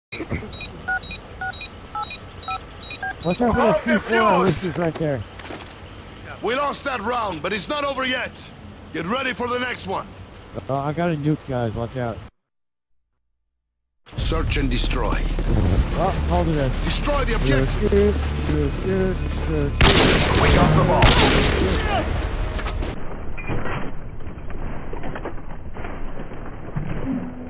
vMCv Gets a Nuke in Search and Destroy!
damn dude